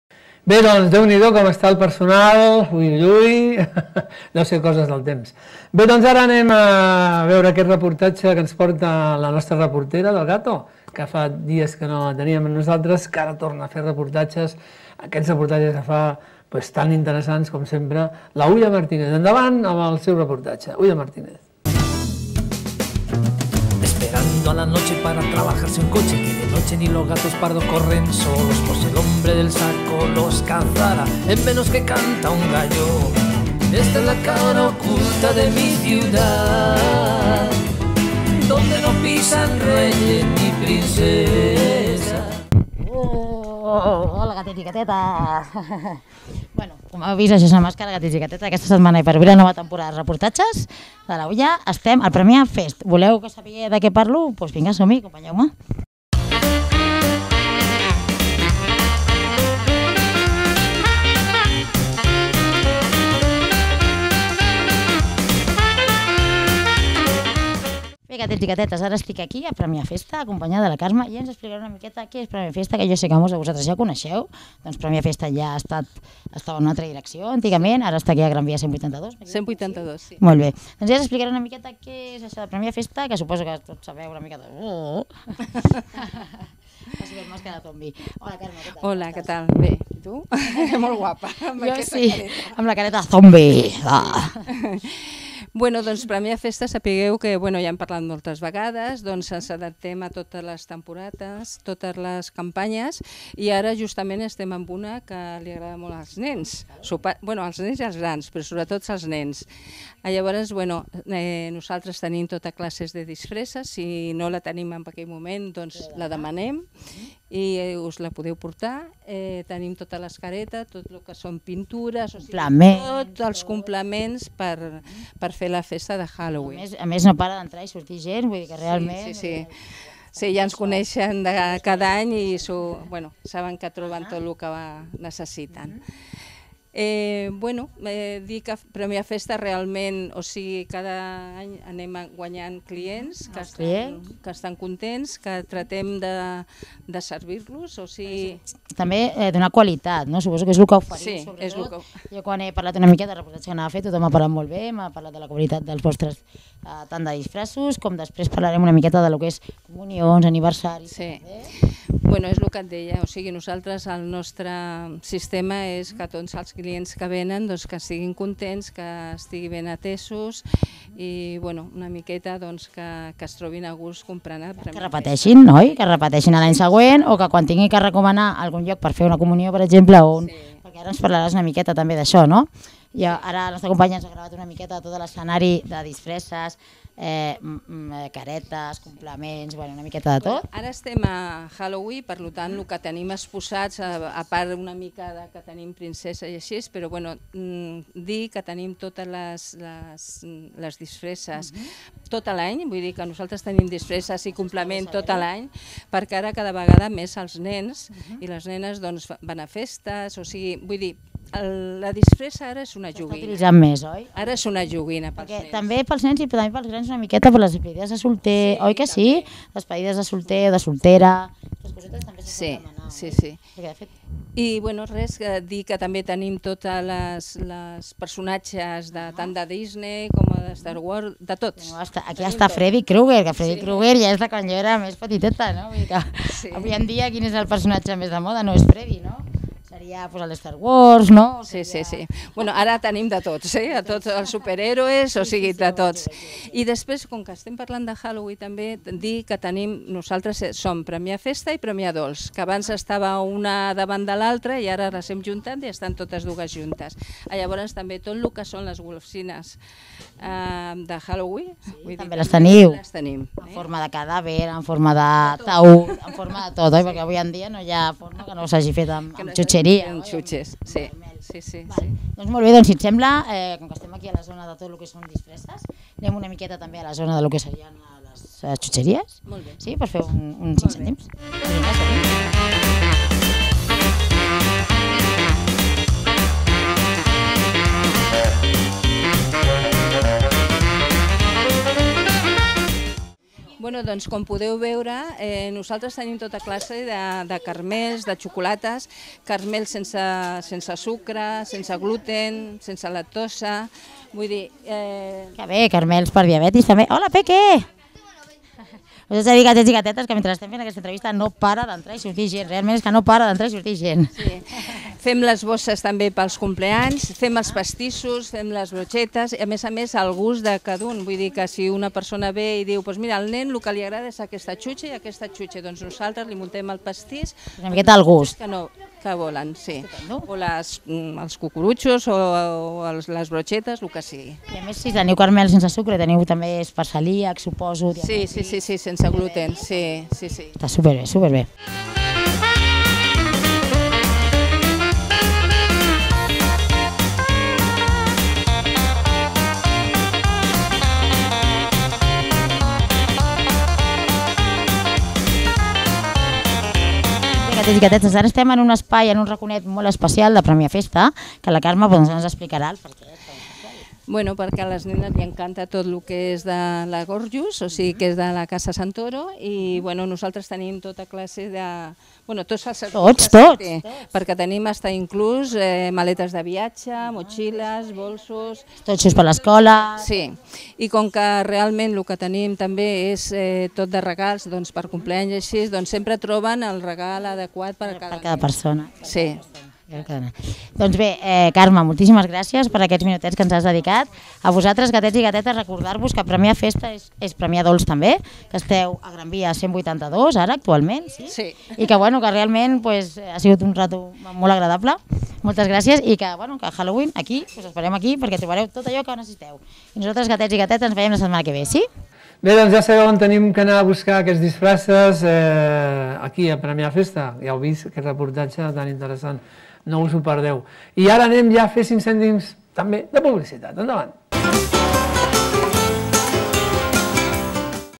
Premià de Festa - Gato Estresao
Este video muestra el reportaje que hicimos sobre una tienda que hay en Premà de Mar en Gran Vía, para uno de nuestros programas llamado el Gato Estresao.